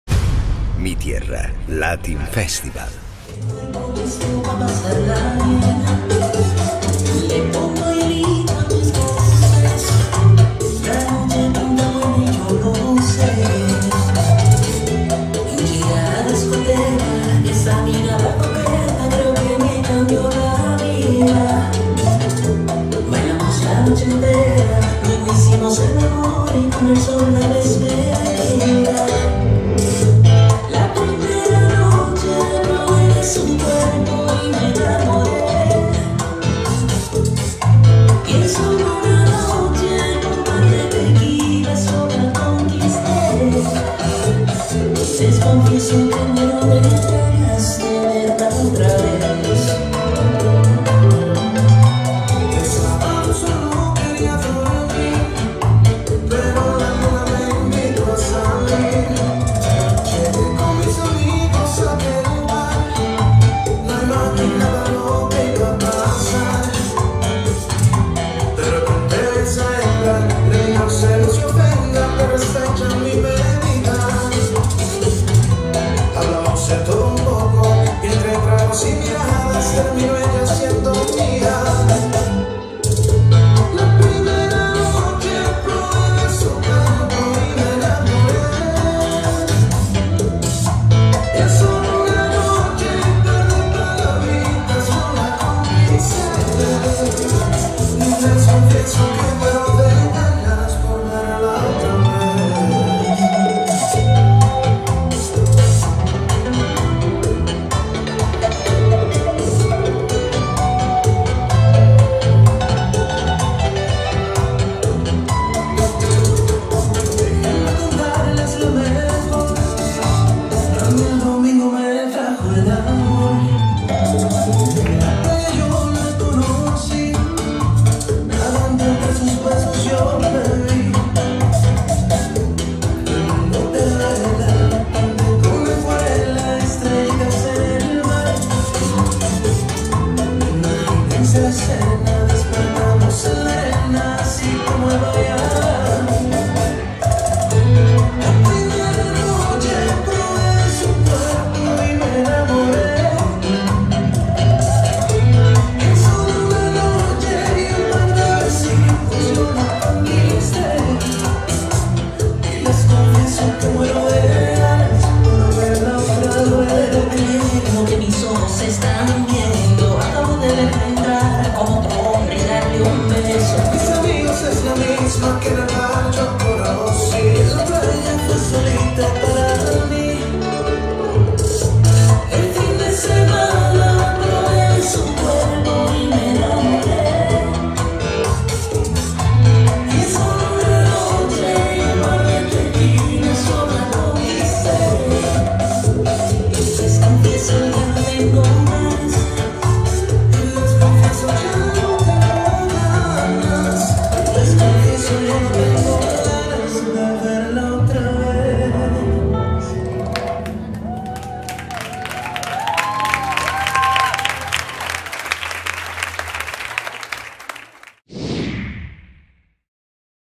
Daniel & Desiree - Bachata Sensual | Mi Tierra Latin Festival 2019
Daniel & Desiree - Bachata Sensual

15-17th November 2019 - Mi Tierra Latin Festival (Cosenza, Italy)
Next Edition:
20-22 November 2020
Mi Tierra Latin Festival 2020